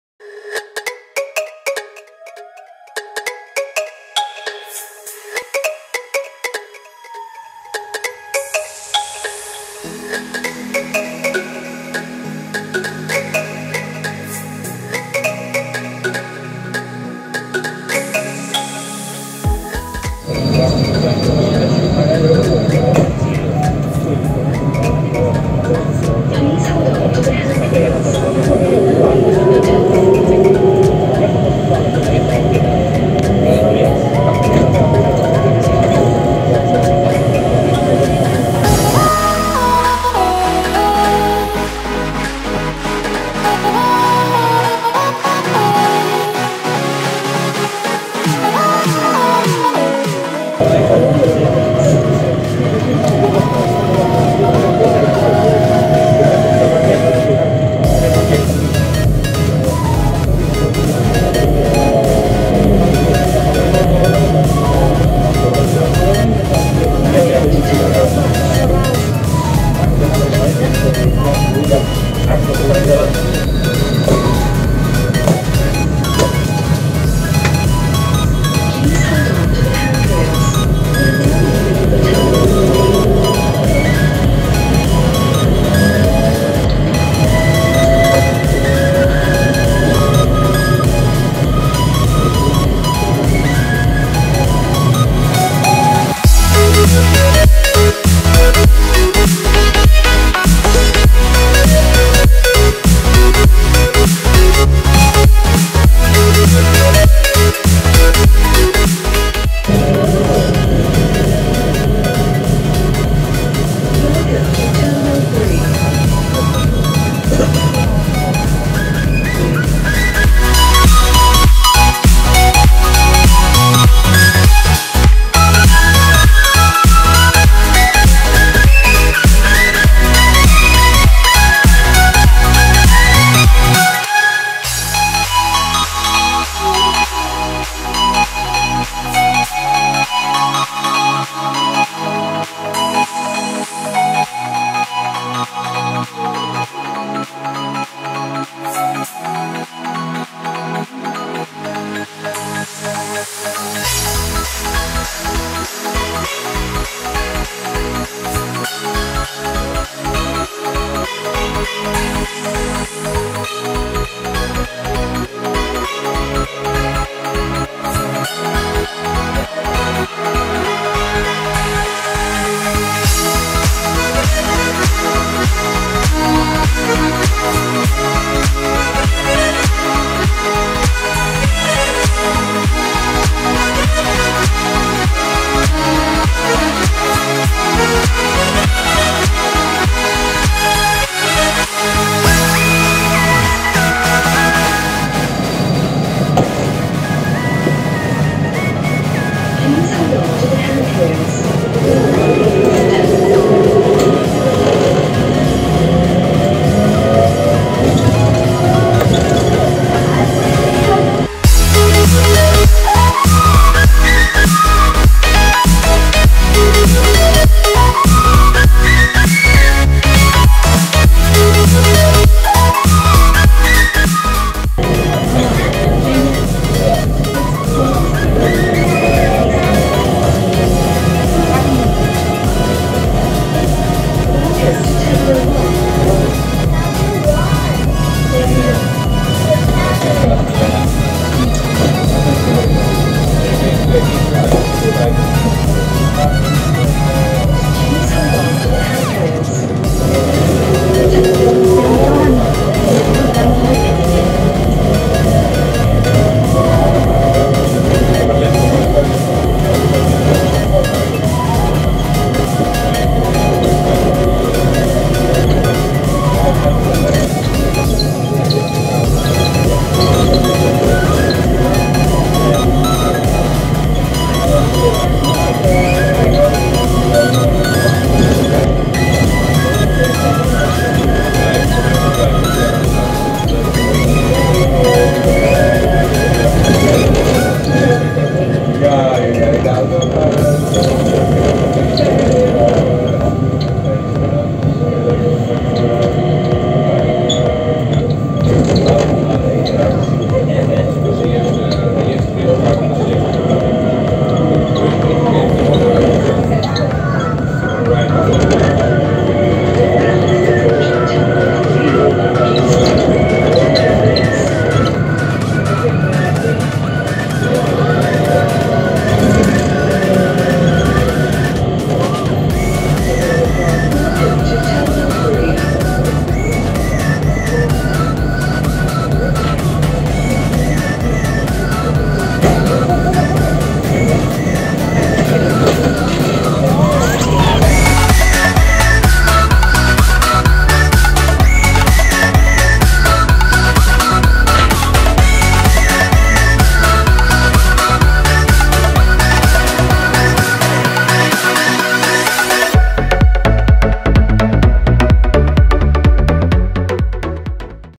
Riding & Trainspotting the Singapore Changi Airport SkyTrain (and Buses)!
So I thought it would be a good idea to ride and trainspot the Changi Airport SkyTrain... (and some Buses in the process)... Sadly only the T1 to T3 and the Gates A to Gates B SkyTrains were open.